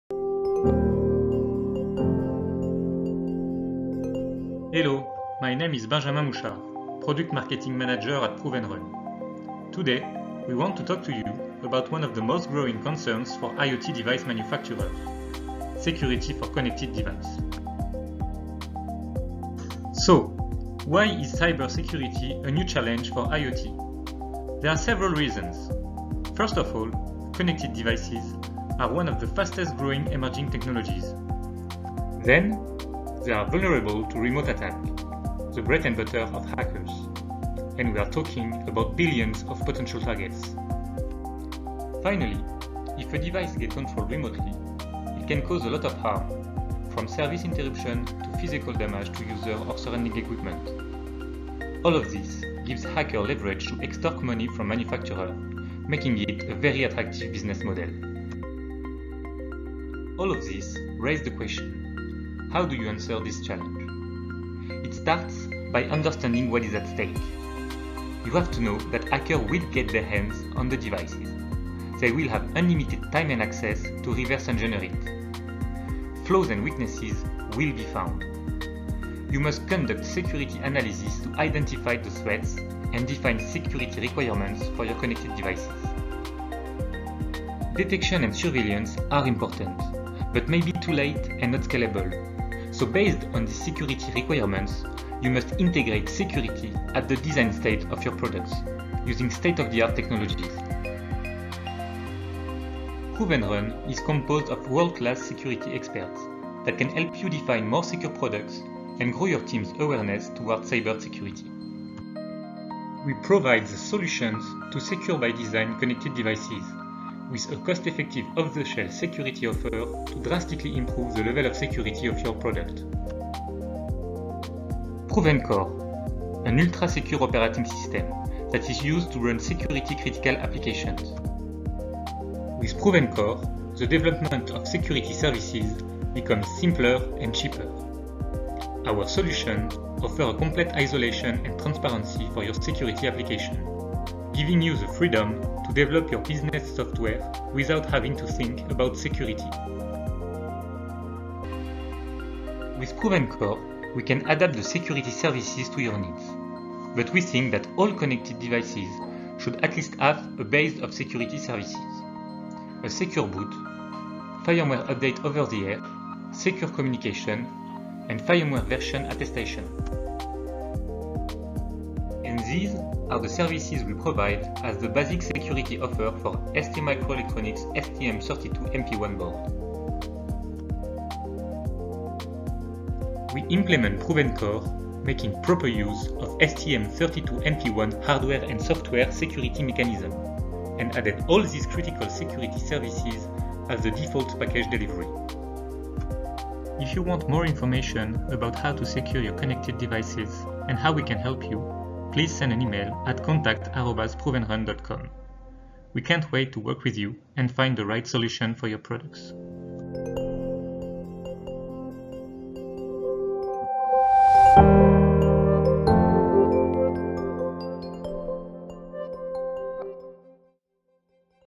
Security for connected devices
Discover why cybersecurity is the IoT’s biggest challenge and find out how to solve this challenge with STM32 MP1.